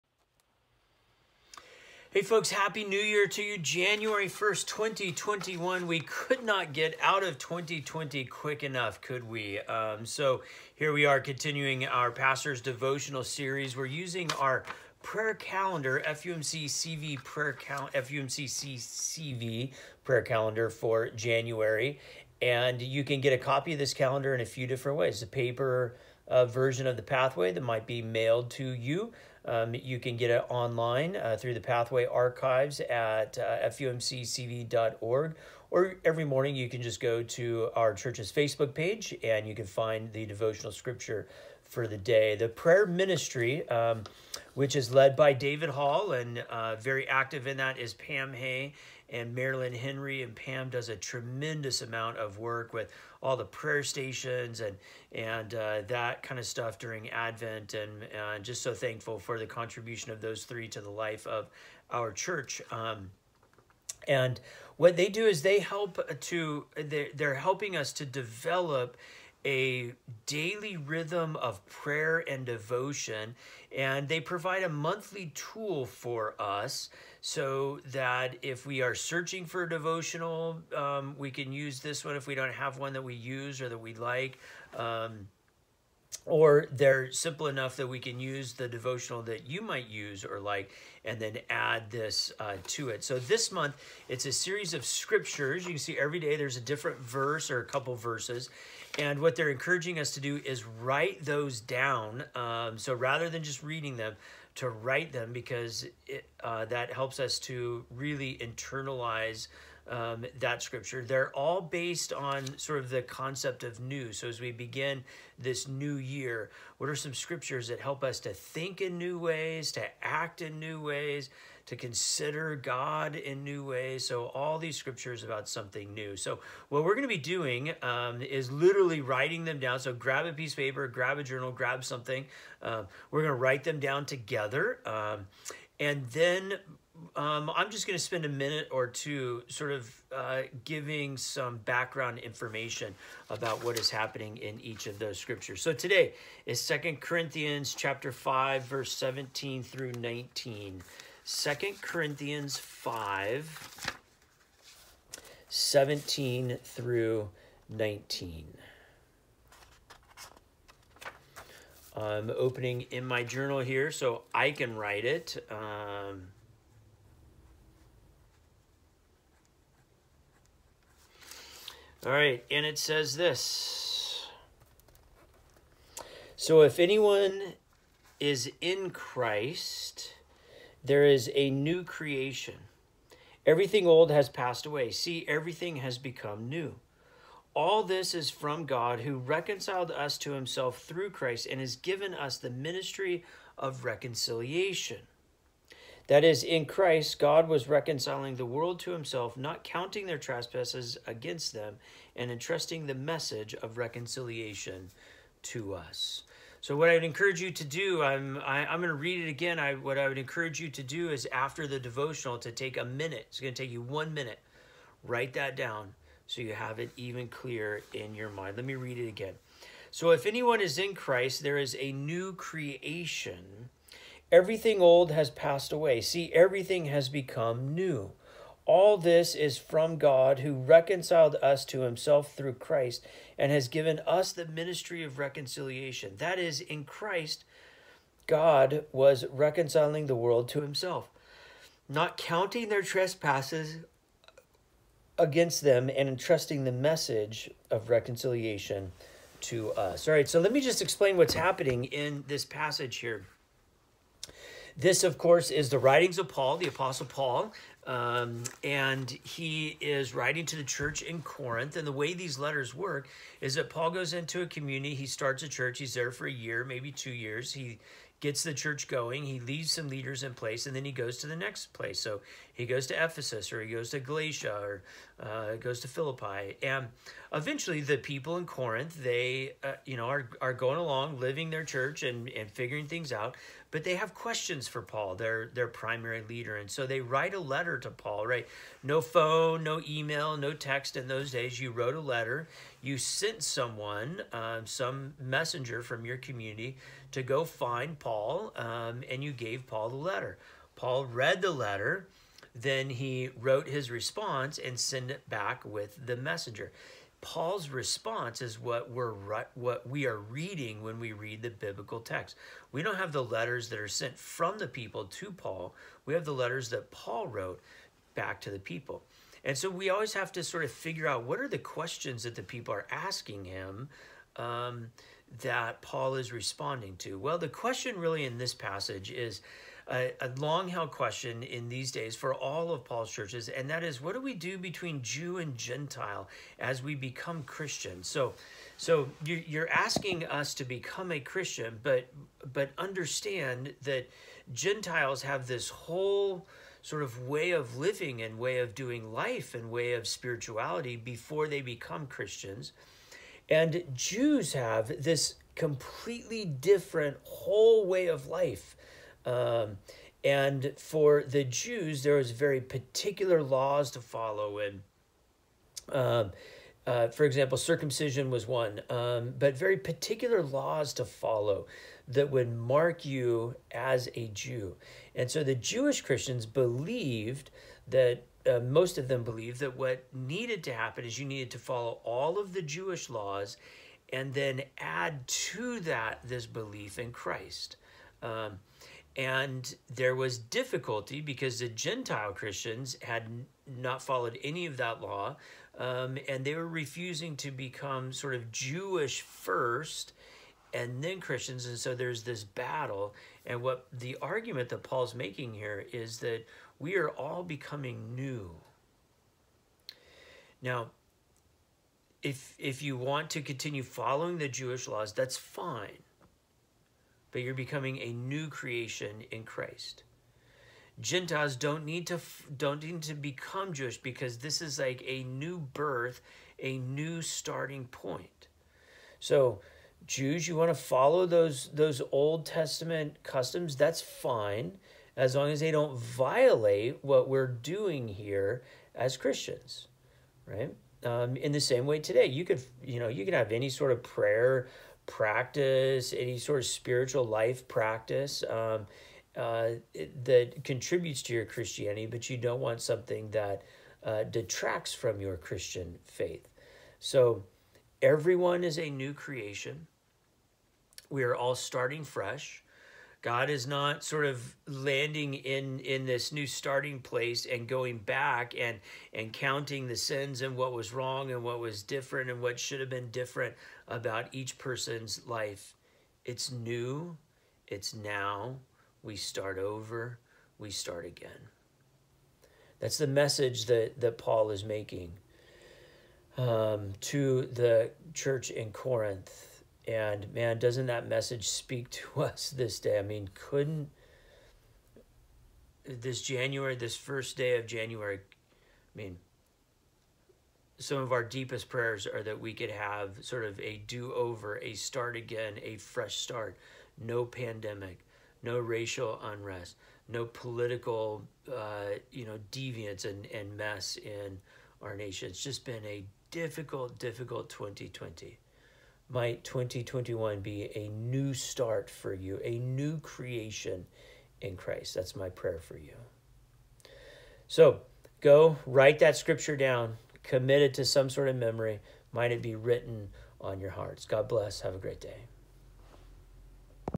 Pastor's Devotional -- January1, 2021 -- 2 Corinthians 5:17-19
Join Pastor Brian as he walks through the January Prayer Calendar created by the Prayer Ministry of FUMCCV. This calendar offers a scripture reading each day that focuses our attention on something new: a new insight, a new way of seeing God, a new way of seeing others. something new with which to begin the New Year. The Prayer Calendar invites us to write the verses down to better remember them and in this devotional video Pastor Brian will briefly cover some background of the day's scripture and also suggest some ways of application. You can access the Prayer Calendar through our or even better through the Church's Facebook page.